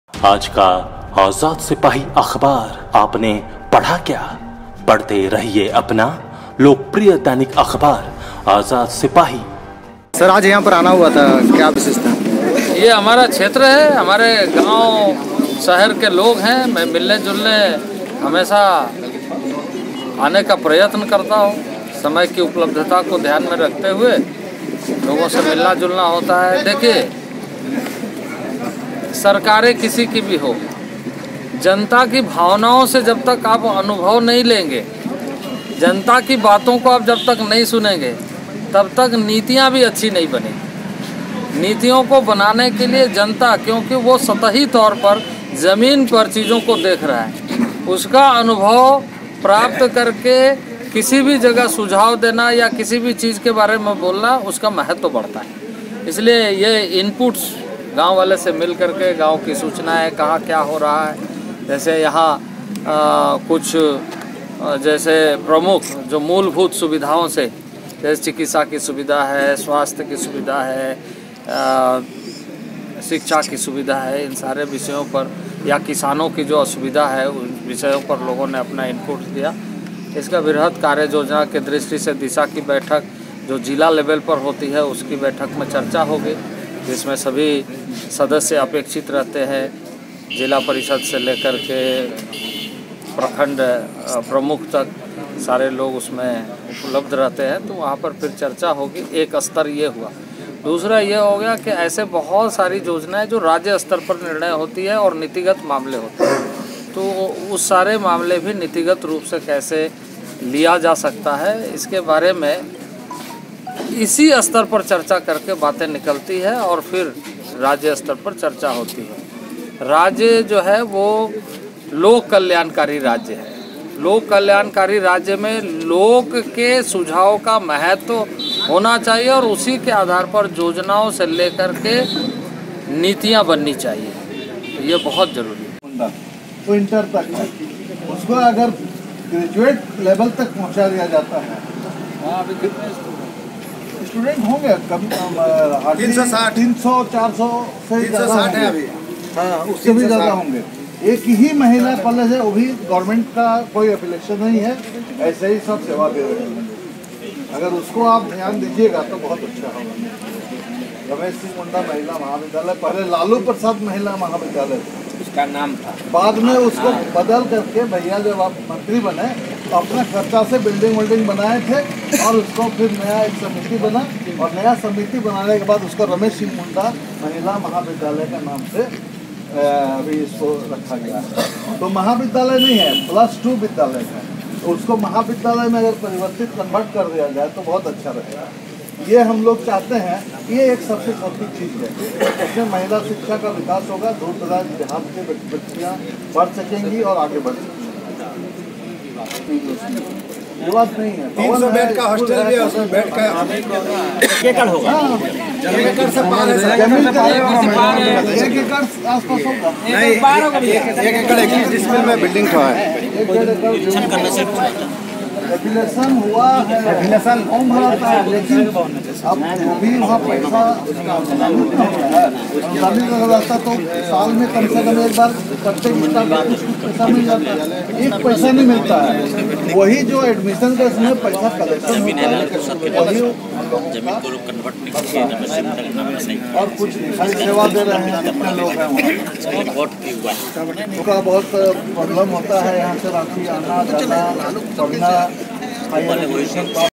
केंद्रीय मंत्री अर्जुन मुंडा बुंडू स्थित कुशवाहा भवन पहुंचे और जनता की समस्याओं को सुना
केंद्रीय मंत्री अर्जुन मुंडा बुंडू स्थित कुशवाहा भवन पहुंचे और जनता की समस्याओं को सुना | आजाद सिपाही